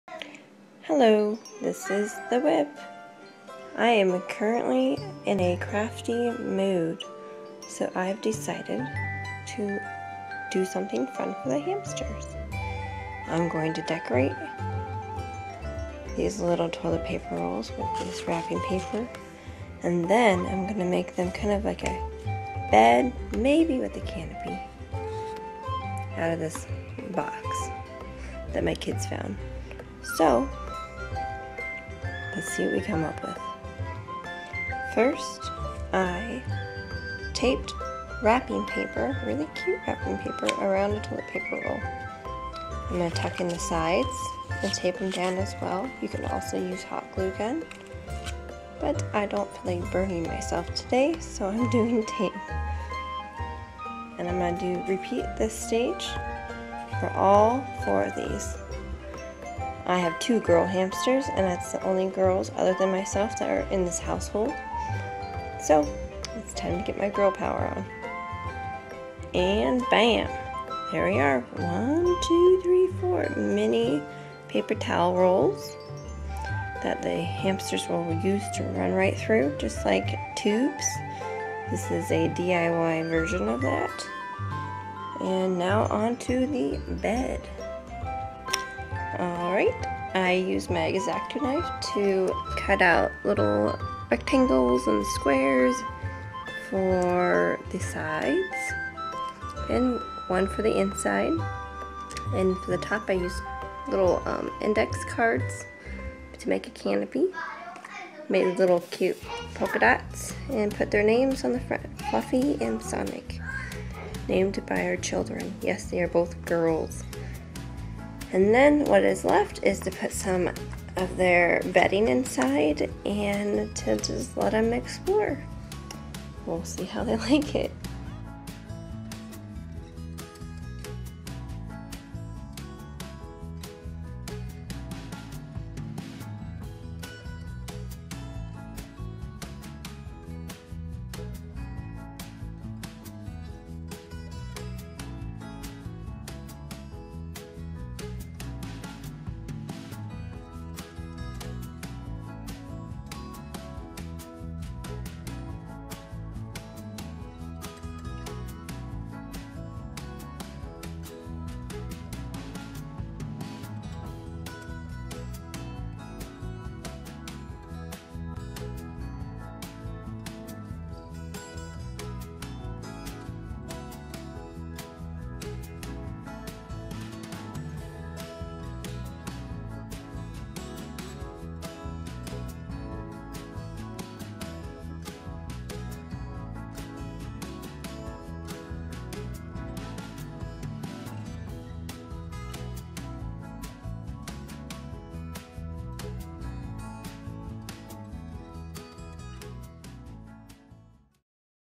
DIY Hamster Tubes
Watch "TheWhip" DIY her own Dwarf Hamster Tunnels and Canopy bed! For those on a budget and limited supplies but want to make something fun for your Hamsters.. this is the place for you!!!